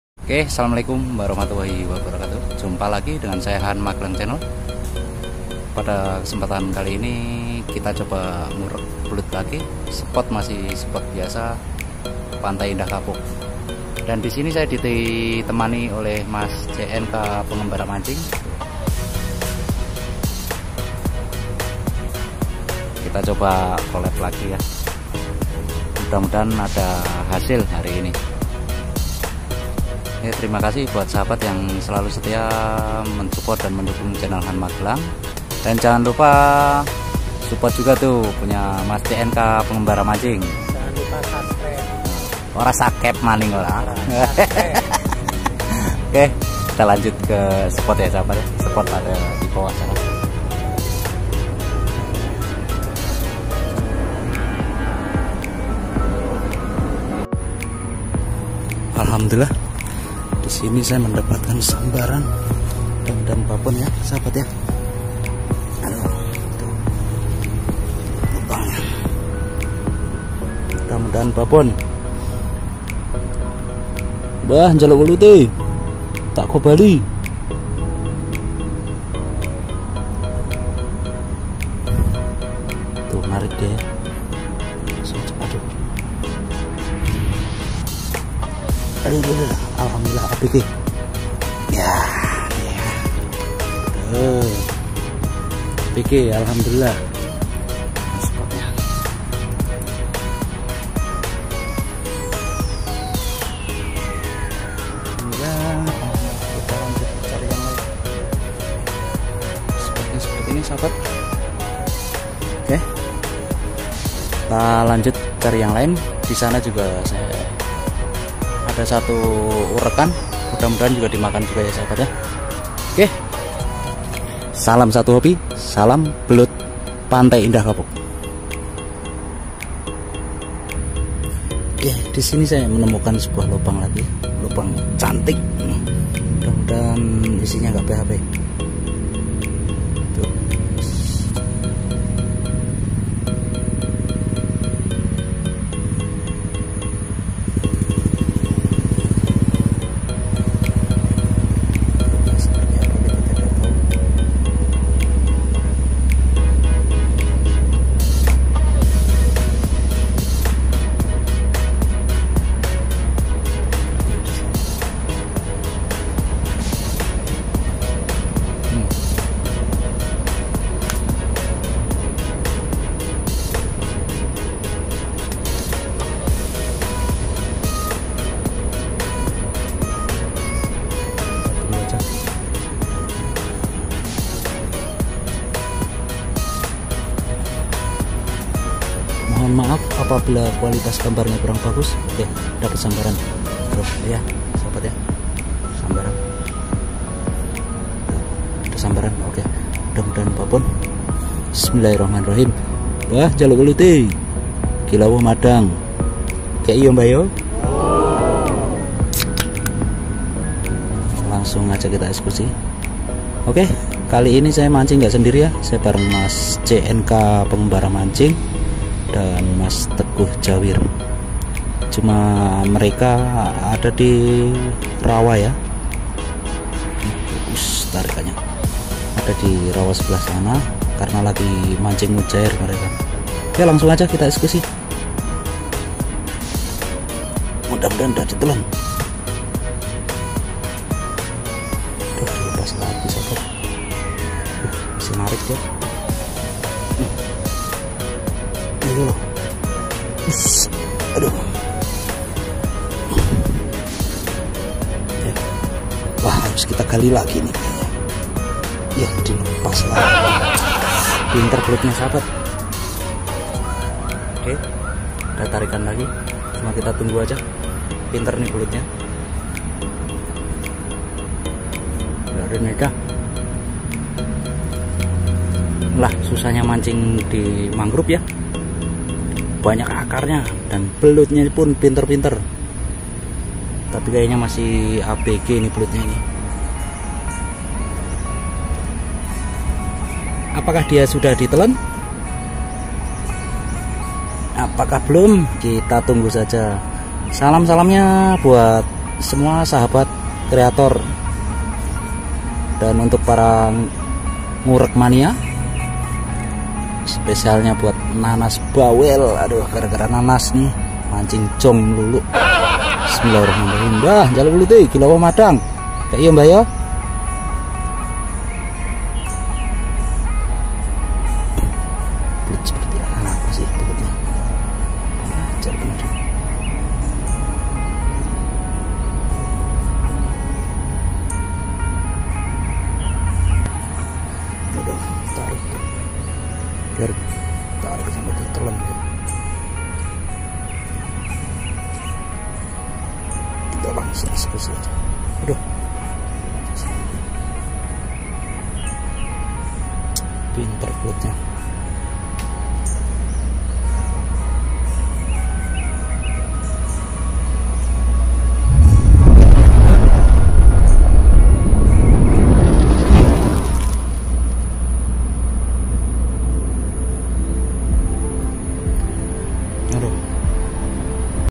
Hampir gak percaya..!! mancing belut di hutan mangrove
Halo Sahabat Dan Teman" semuanya kali ini saya coba mancing belut lagi di spot liar jakarta utara tepatnya kawasan ekowisata mangrove pantai indah kapuk ( PIK ), dan di sini juga cocok untuk berlibur / wisata bersama dengan keluarga sambil mancing, seperti apa keseruannya yuk kita simak bersama-sama

Terimakasih Dan Selamat Menonton...